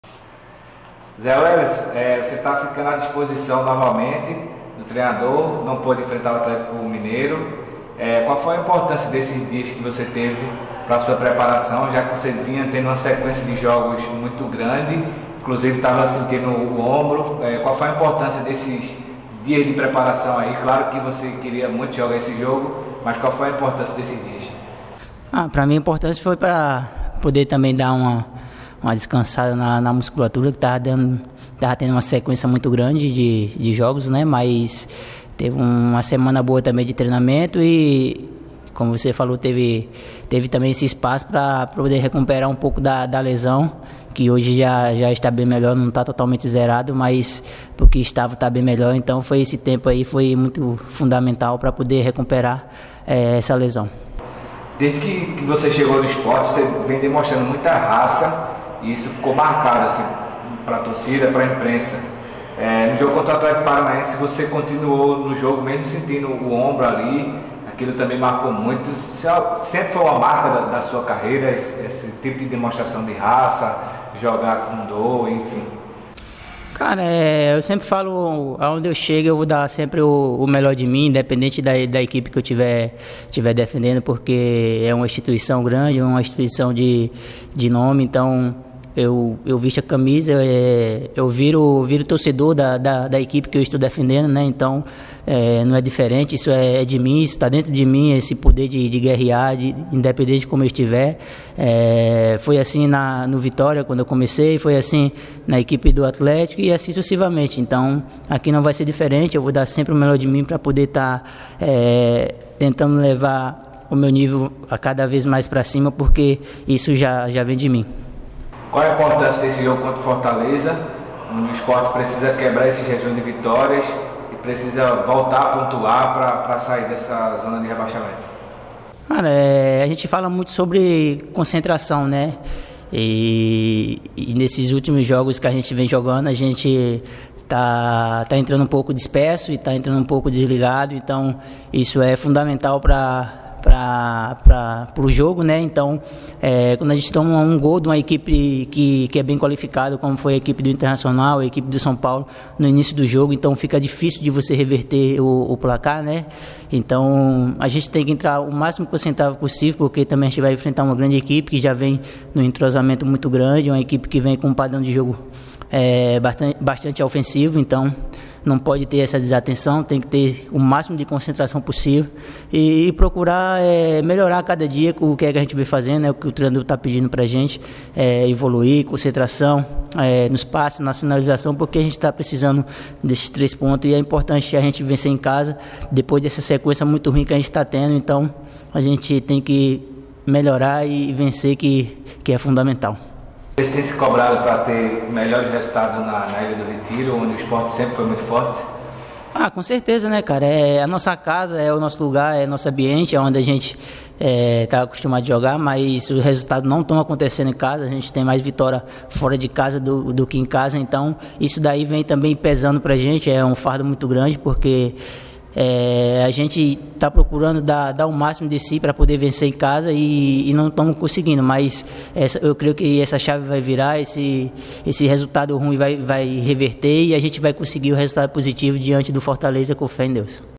Zé Welison retorna ao time e fala sobre jogo diante do Fortaleza
O volante Zé Welison retorna ao time e projeta partida contra o Fortaleza, a ser realizada neste domingo (26), pelo Campeonato Brasileiro. Assista.

Imagens: Thiago Lemos